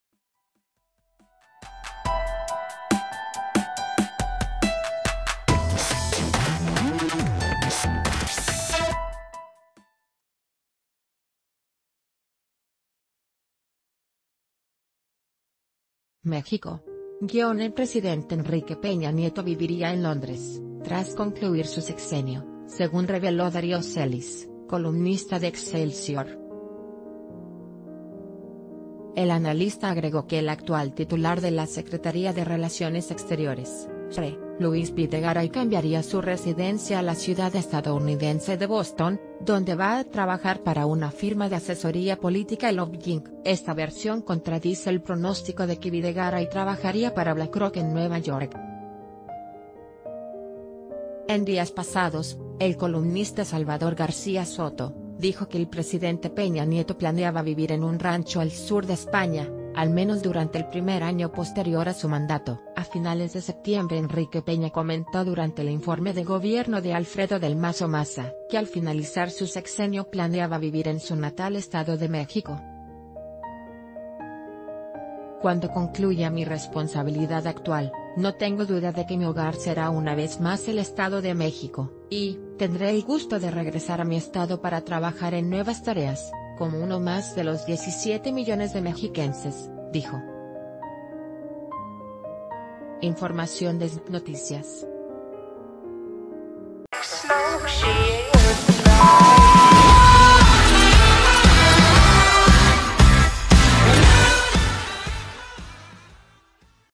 EPN se mudará a Londres tras su mandato - El Diario de Yucatán
MÉXICO.- El presidente Enrique Peña Nieto viviría en Londres, tras concluir su sexenio, según reveló Darío Celis, columnista de Excélsior. El analista agregó que el actual titular de la Secretaría de Relaciones Exteriores (SRE), Luis Videgaray cambiaría su residencia a la ciudad estadounidense de Bo...